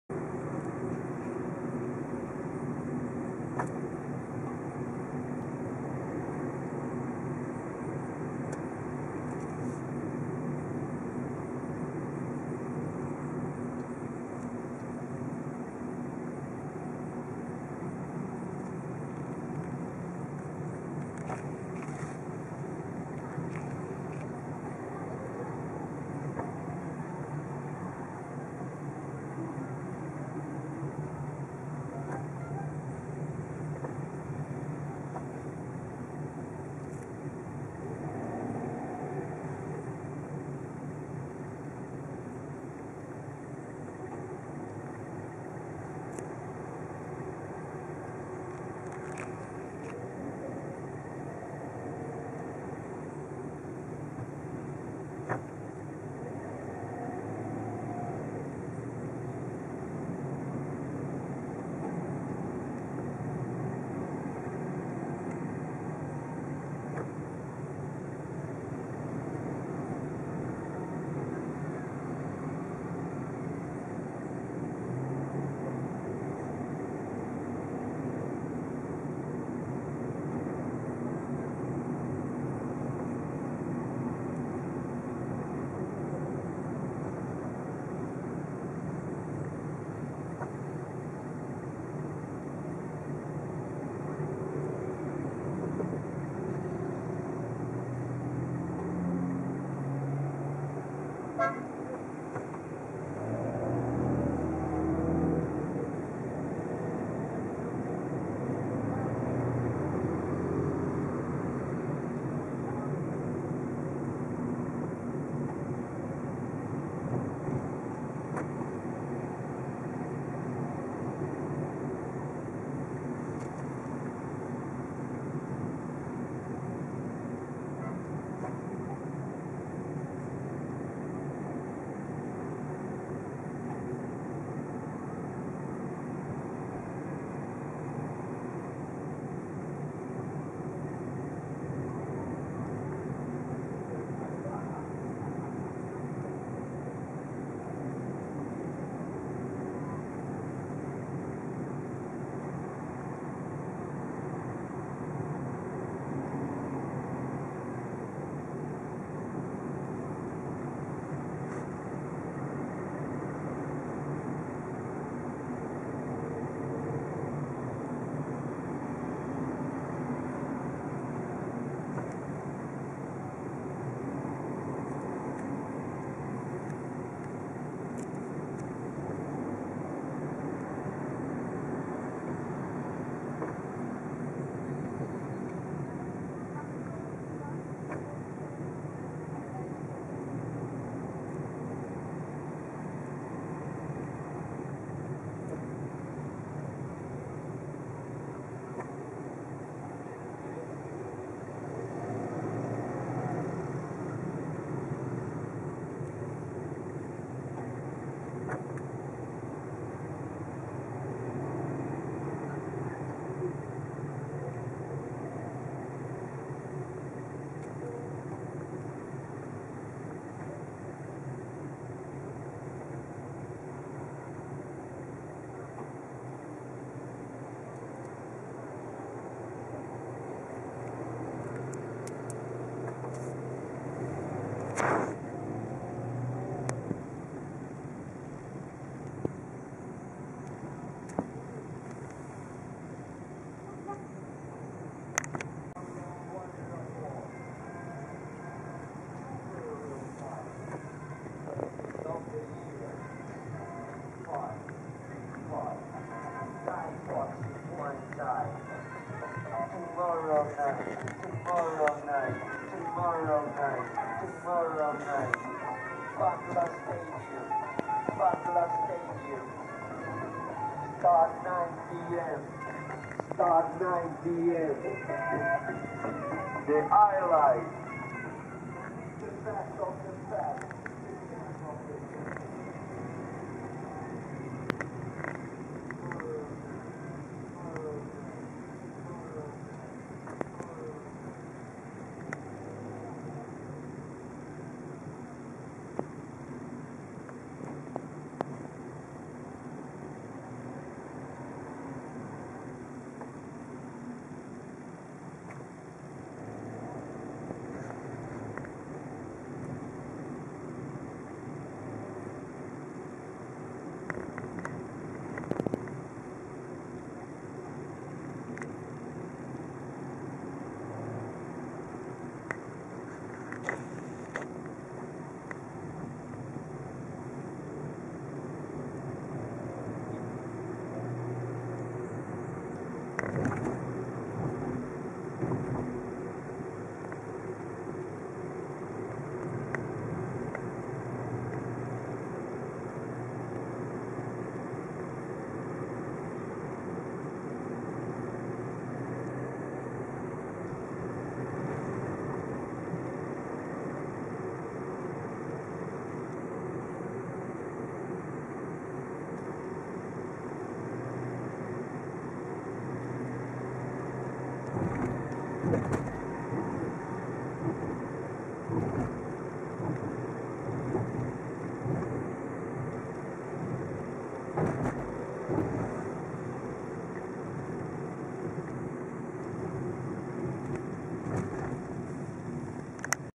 Patong Beach Road Phuket Thailand
A drive down Patong Beach Road Phuket Thailand.